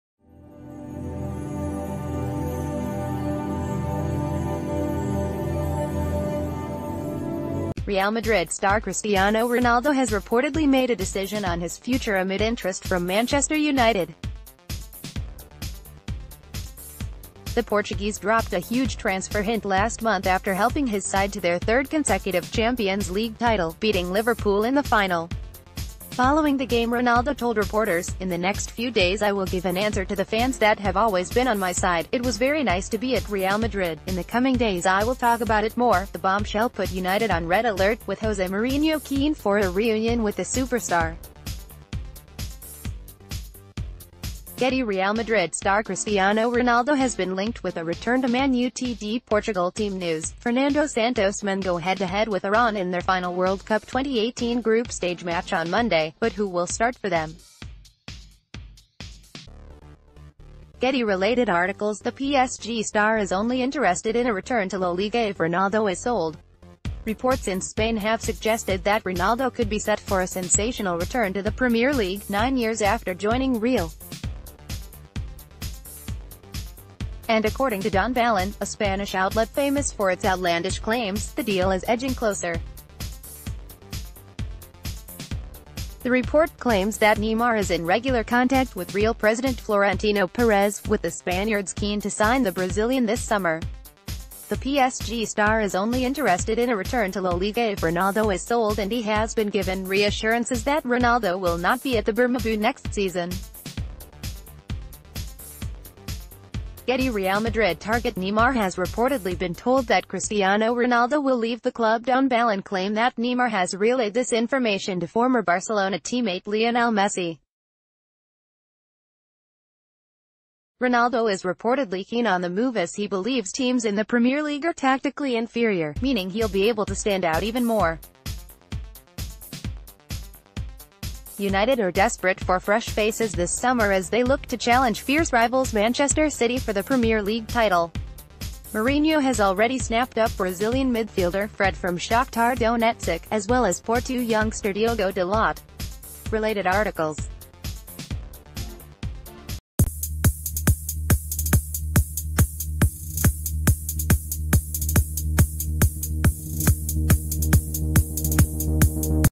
Breaking News | Man Utd transfer news: Neymar 'tells' Lionel Messi of Cristiano Ronaldo Real Madrid
Breaking News | Man Utd transfer news: Neymar 'tells' Lionel Messi of Cristiano Ronaldo Real Madrid plans

REAL MADRID star Cristiano Ronaldo has reportedly made a decision on his future amid interest from Manchester United. The Portuguese dropped a huge transfer hint last month after helping his side to their third consecutive Champions League title, beating Liverpool in the final. Following the game Ronaldo told reporters: “In the next few days I will give an answer to the fans that have always been on my side."It was very nice to be at Real Madrid, in the coming days I will talk about it more."The...